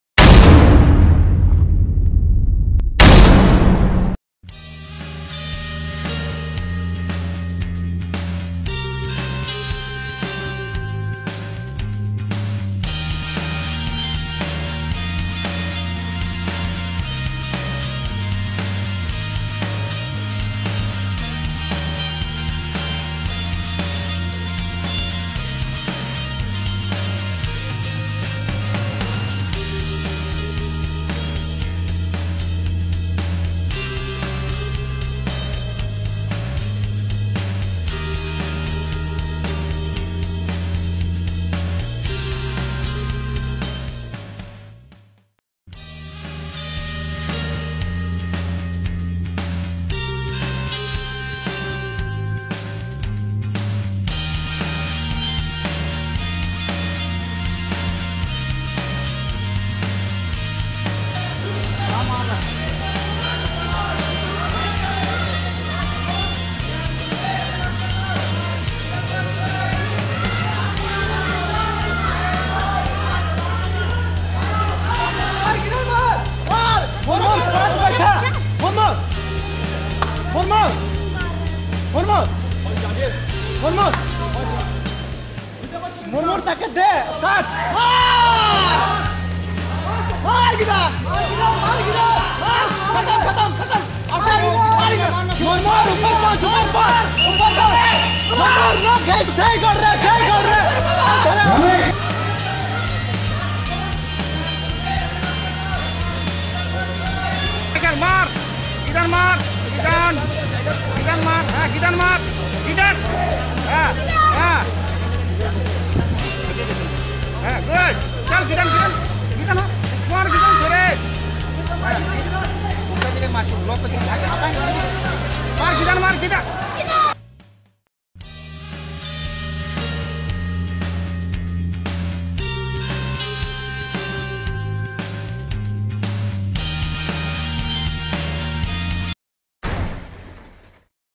MAS OYAMA CHALLENGE CUP
All India open full contact karate championship 2017 .4th&5th march at purulia w.b .sensai-supratim mitra ( 3rd dan black belt ) tournament organizing committee & sensai sonu pandey (2nd dan black belt ) convenor...tournament organizing committee ...